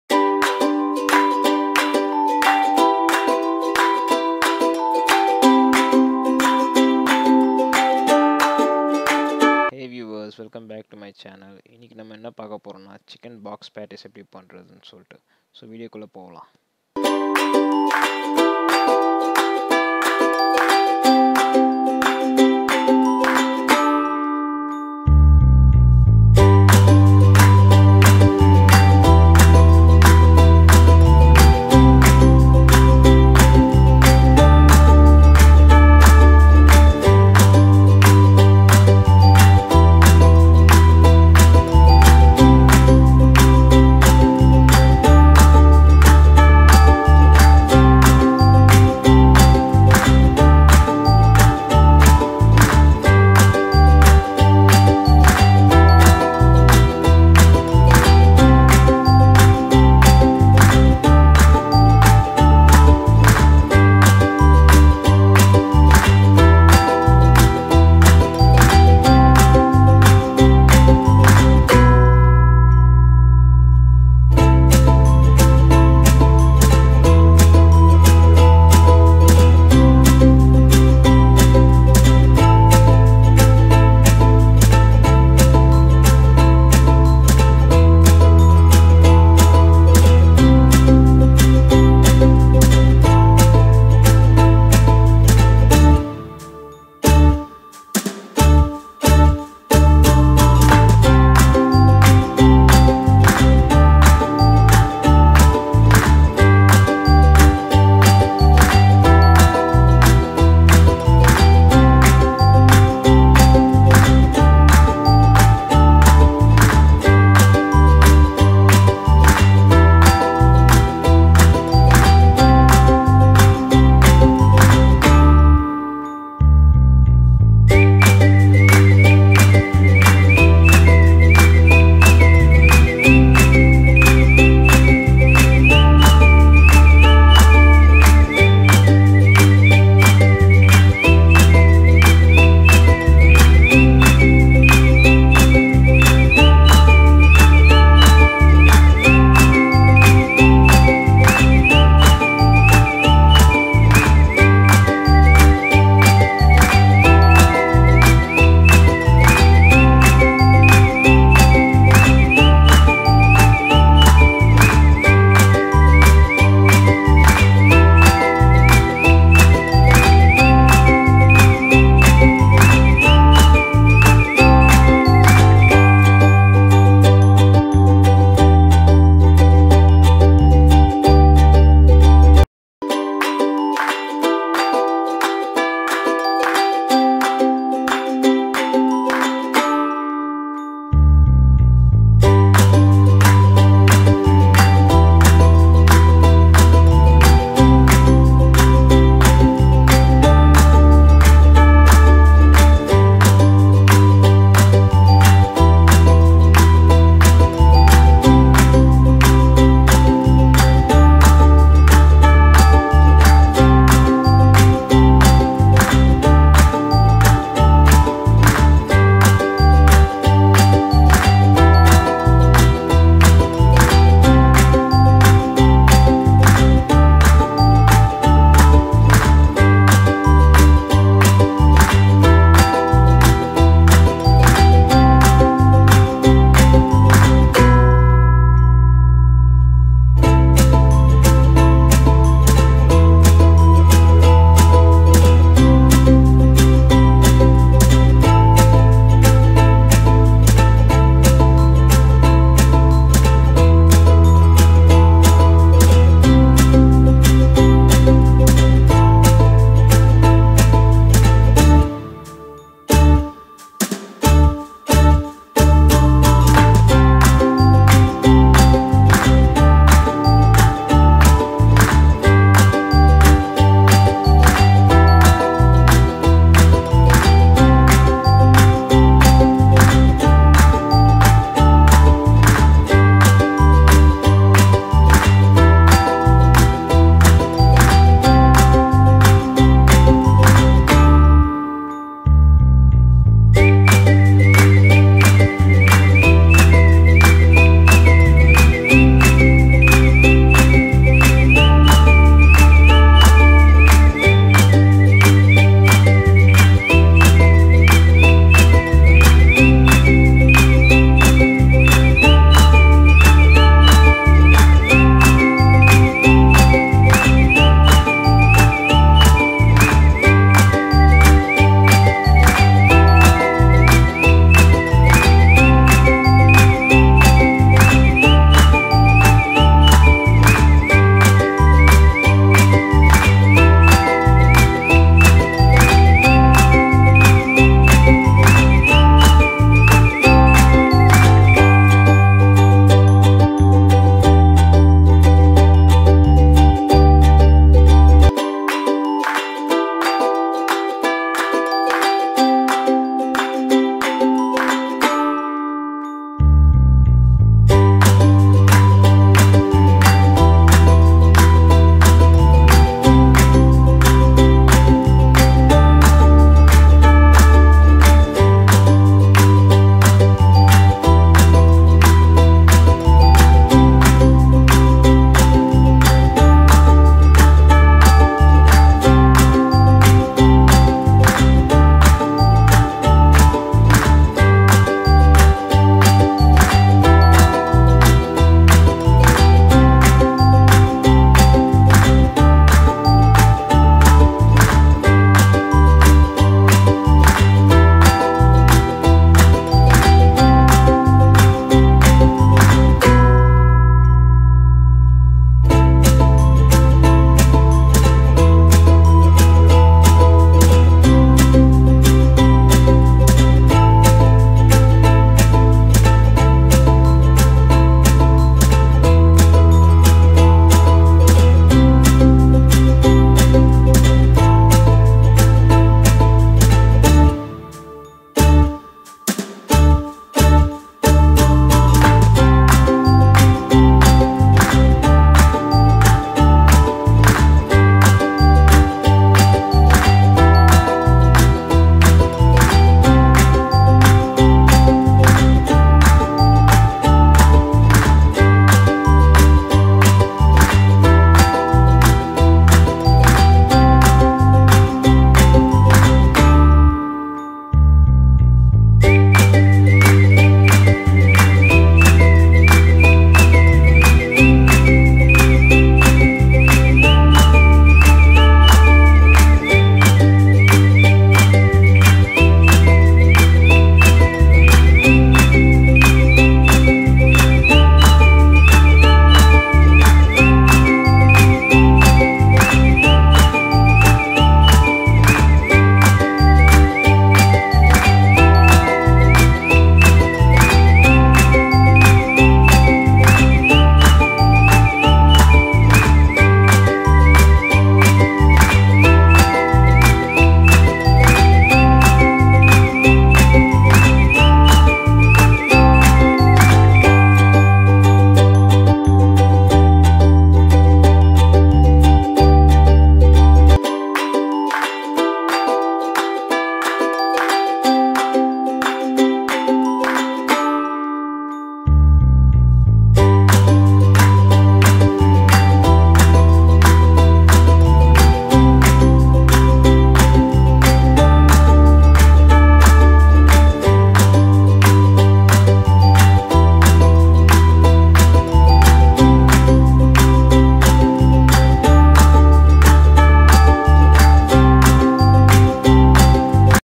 Tasty Chicken Box Patties
Hi Viewers,

Chicken Box Patties is a mouthful feast, you will love eating all day,

So Try our receipe, also do comment in the comment section